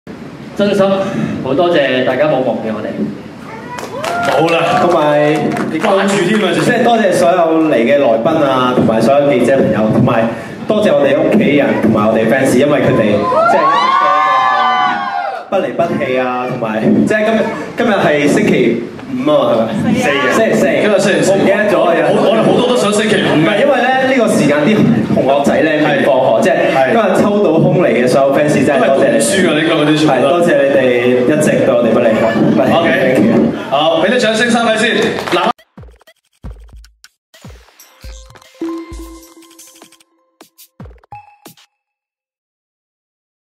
天堂鳥簽新東家 多謝大家不離不棄
兩年前與舊東家鬧合約糾紛的男子組合BOP（天堂鳥），昨日與新公司天盟娛樂舉行加盟簽約儀式，三子Tyrese、Gordon、Lincoln表示已跟舊公司冰釋前嫌達成和解，一切已搞妥感到很開心，展望將來不想再提以前的事，加盟新公司後會加強香港宣傳。